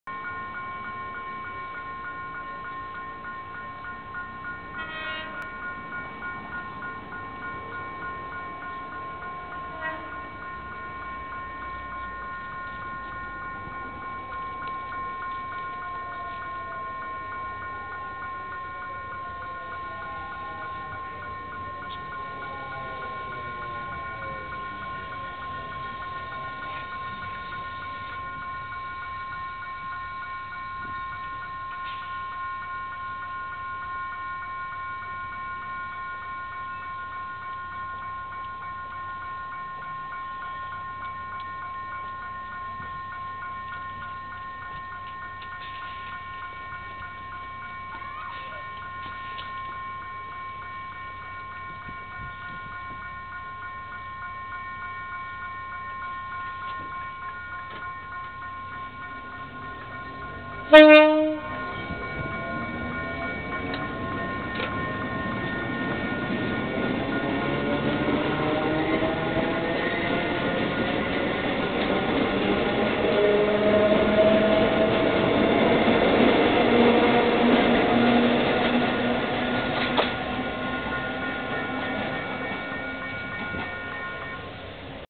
Toshibas en la estación Doctor Francisco Beiró (5)
524 - 15 de julio de 2013 - METROVÍAS/FCGU. Formación Toshiba 4 (MU 3819, MU 3871, MFU 3007, MU 3811, MU 3816 y MU 3868) arribando y partiendo de la estación Doctor Francisco Beiró rumbo a General Lemos.